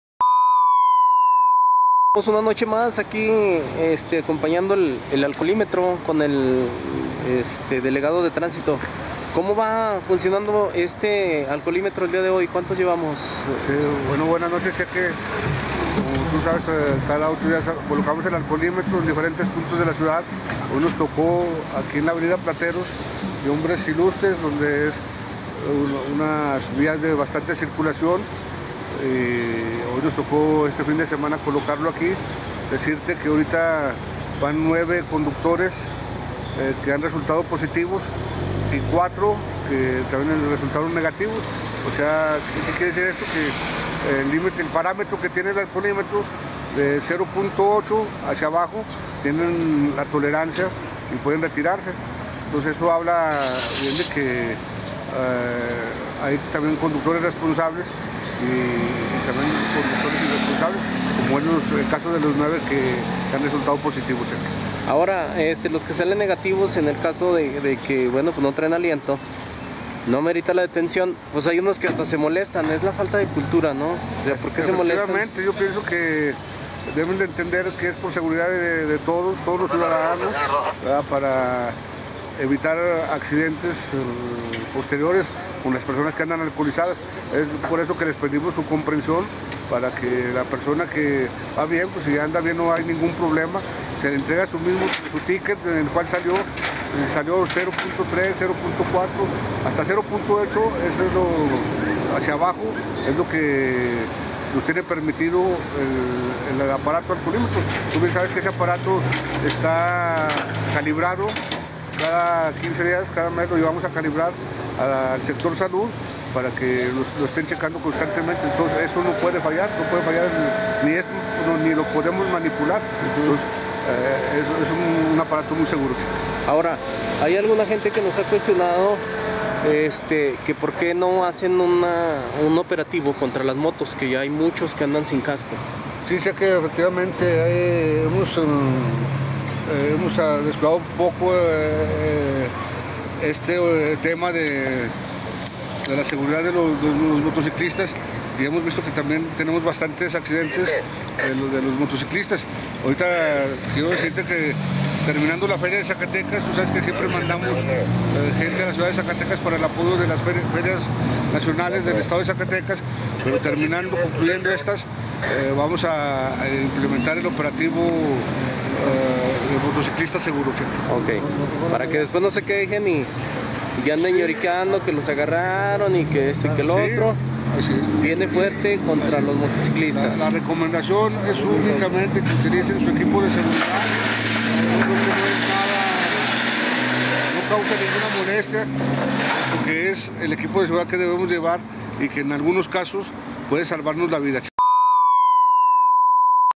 Operativo alcoholímetro
Como cada fin de semana se llevó a cabo el alcoholímetro a cargo de Tránsito del Estado, así mismo dieron recomendaciones para prevenir accidentes. También mencionaron que pronto se realizará un operativo de motociclista seguro.